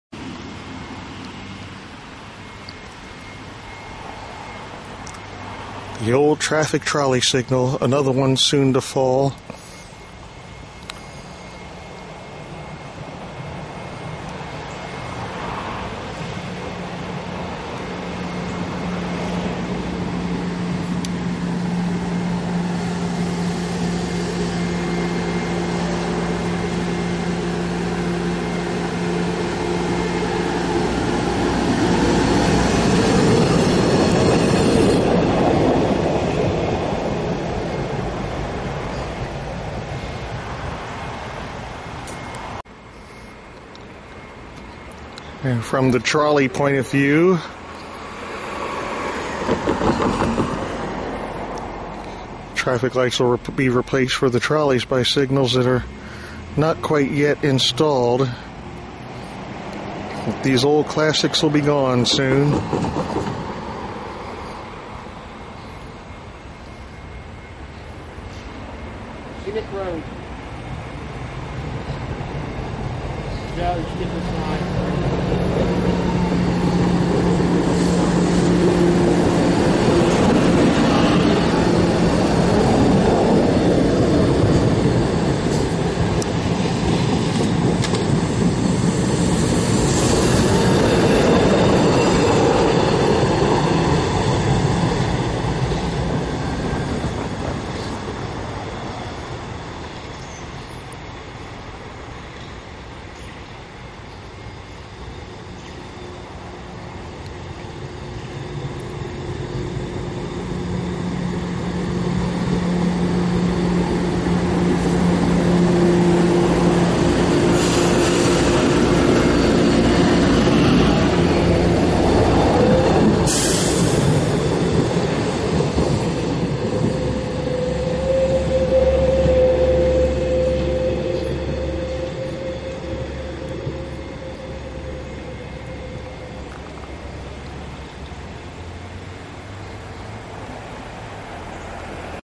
Trolley Traffic Lights On Borrowed Time
On the Media and Sharon Hill lines, the recent construction includes new trolley and traffic signaling at grade crossings.  In some places this will replace traditional railroad gates and crossings.  In others, it will replace the old trolley/auto traffic lights like this one at the crossing of the trunk line and Irvington Road.

Here, we see the light in action, both from the auto and trolley perspective.